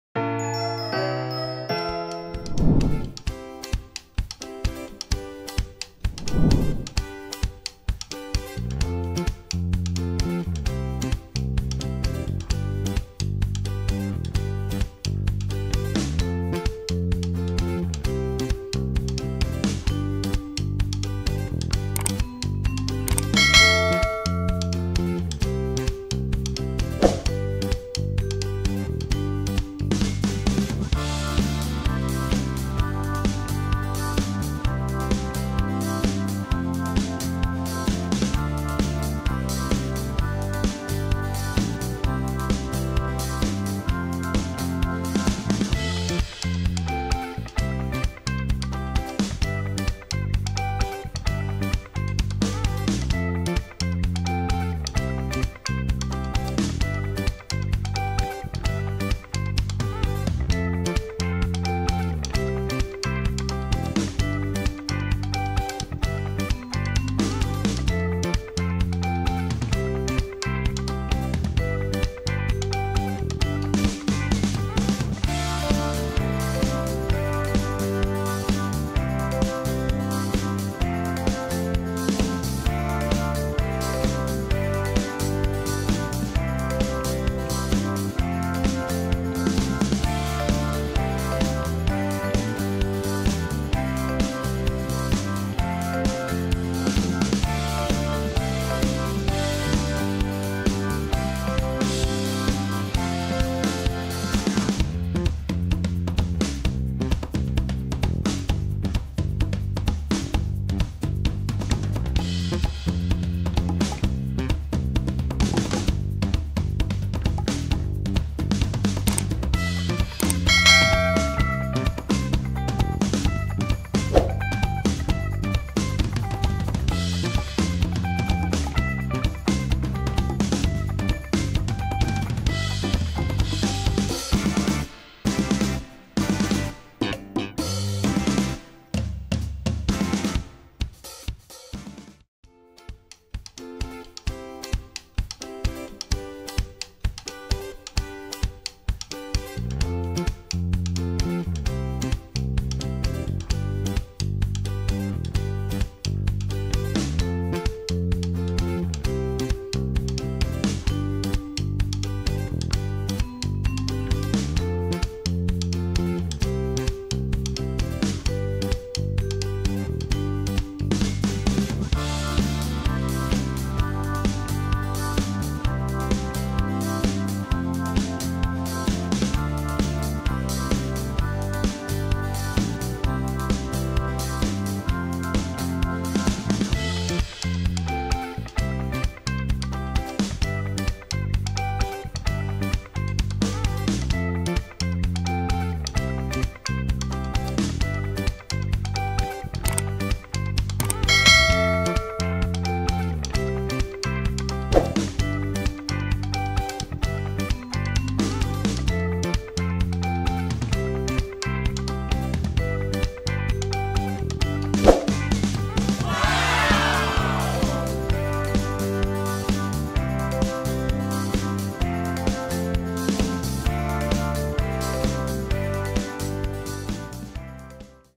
Garten of BanBan CHAPTER 3 Coloring pages How to paint All New monsters
Hi !  In this video I paint GARTEN OF BAN BAN   ALL  NEW  MONSTERS coloring pages !!